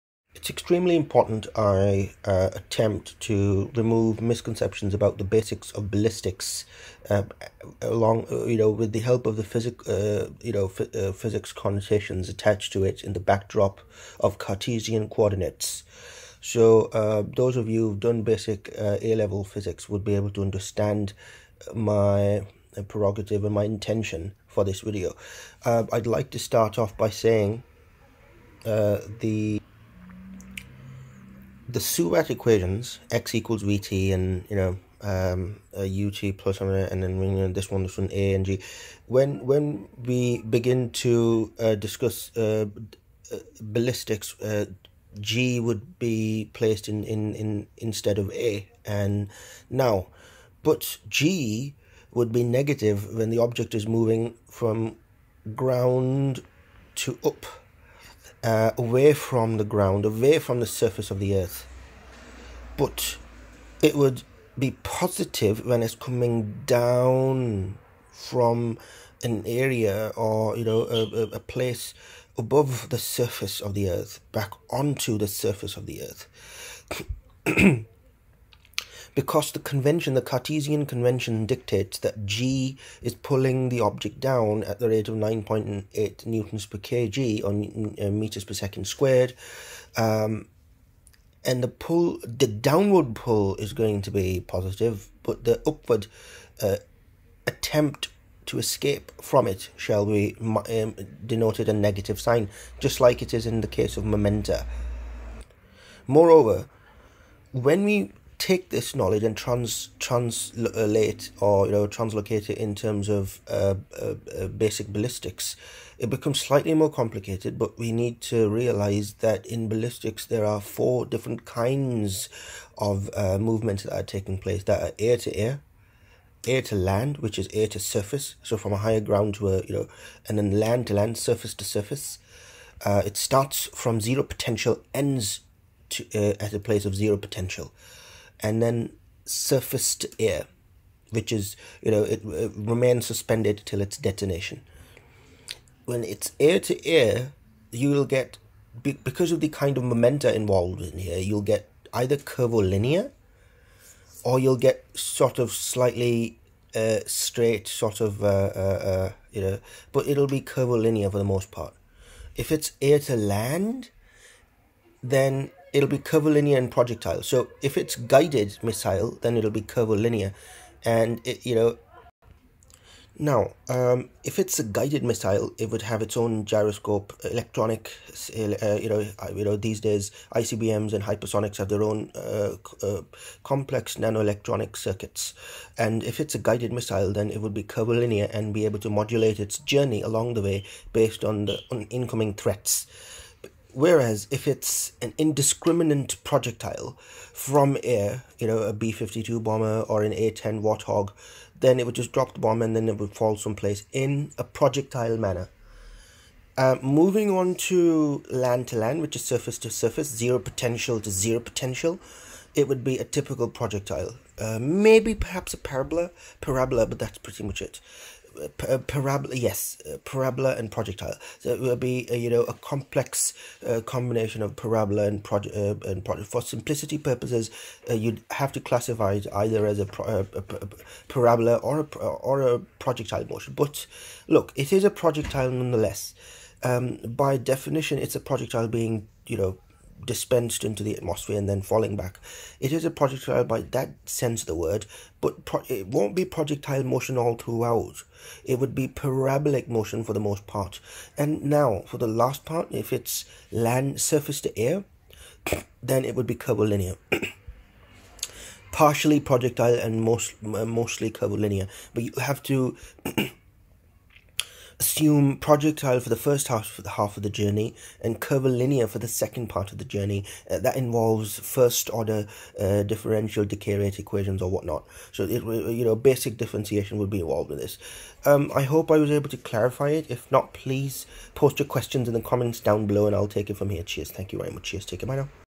Ballistics - Misconceptions
An attempt to remove misconceptions in basic understanding of ballistics.